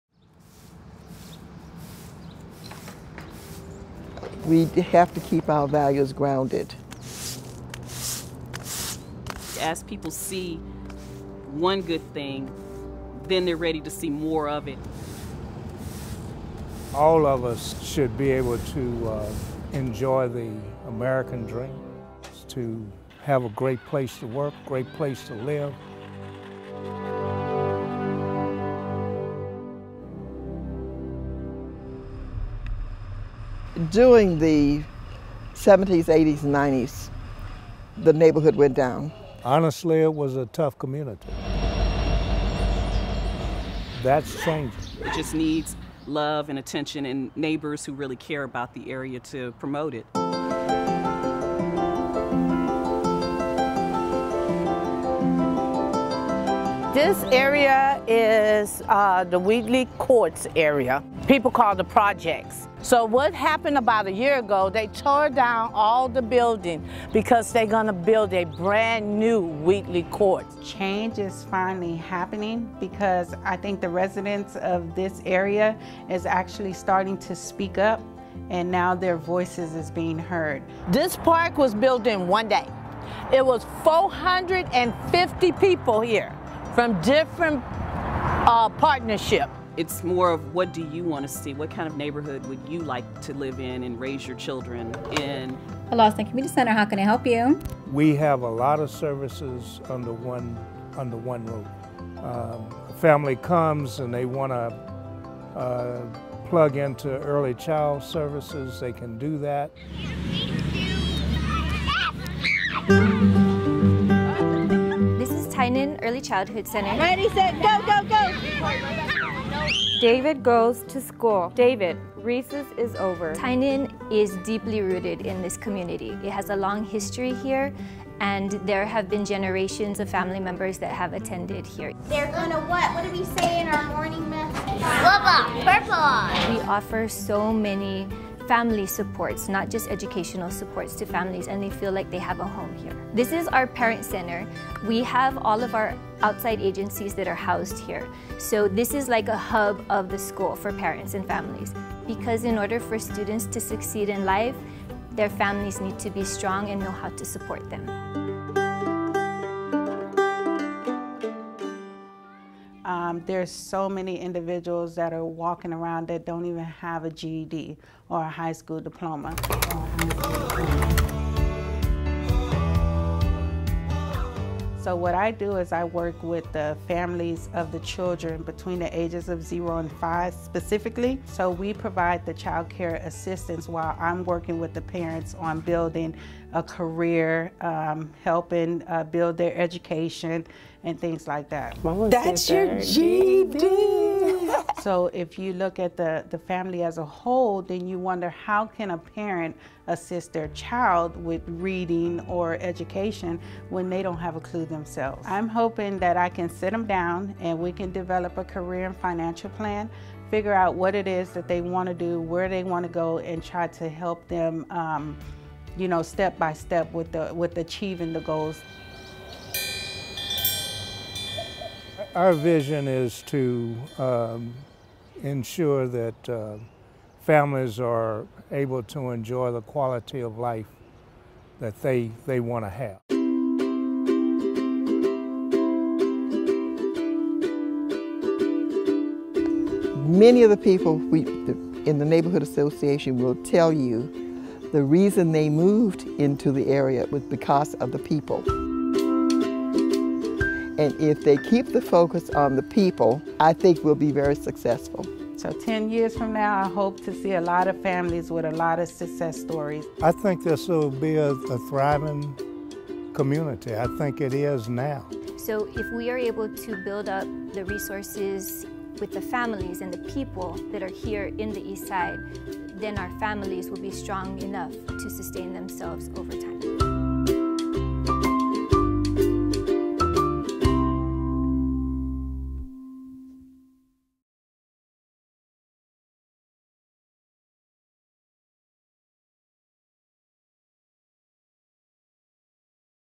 Welcome to EastPoint (San Antonio, Texas)
San Antonio's EastPoint is one of three neighborhoods where the Foundation is partnering with a local community change initiative focused on transforming the neighborhood into a better place for kids and families to live, work and grow. Learn more about EastPoint from the people who call it home — and the vision they have for their community's future.